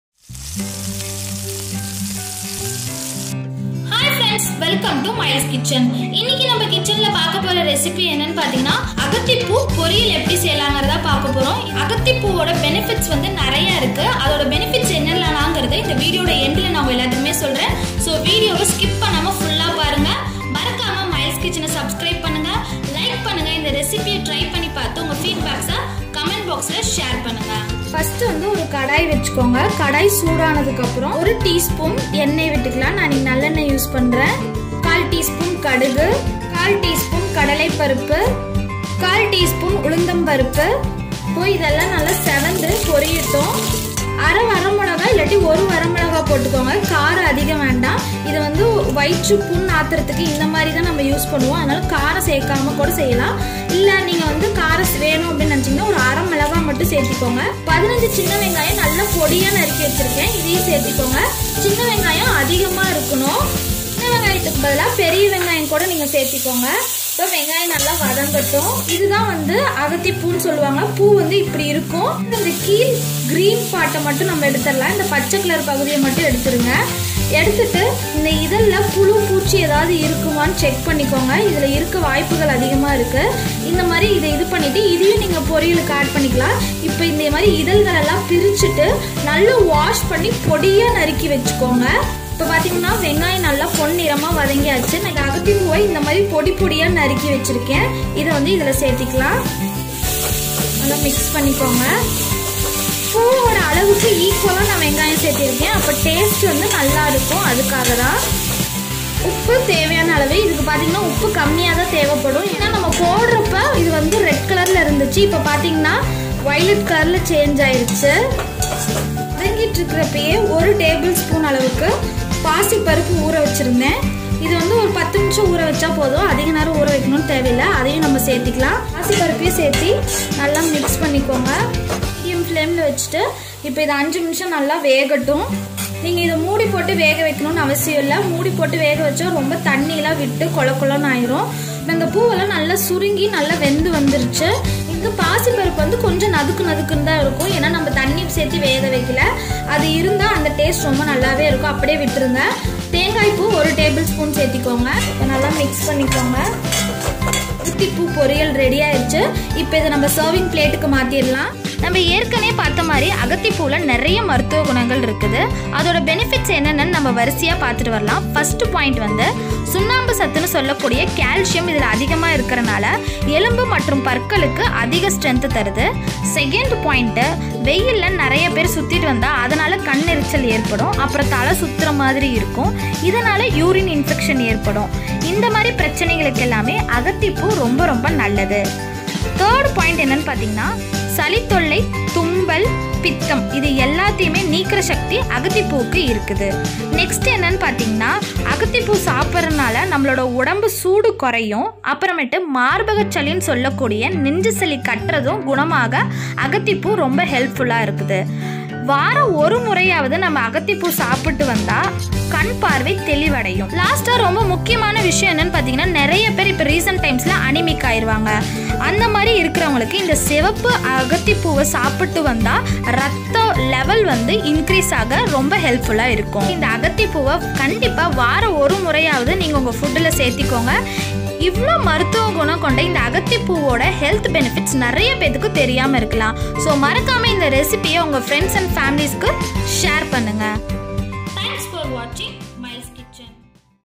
AgathiPoo Poriyal in Tamil | Hummingbird Flower Stir Fry | Red AgathiPoo Poriyal | அகத்தி பூ பொரியல்
Ingredients :

1cup Hummingbird Flower / Agathi Poo
15 Shallots
1/2 Red Chilli
1tbsp Moong Dhal
1tbsp Grated Coconut
1/4tsp Mustard Seeds
1/4tsp Chana Dhal
1/4tsp Urad Dhal
Salt to taste
Gingelly Oil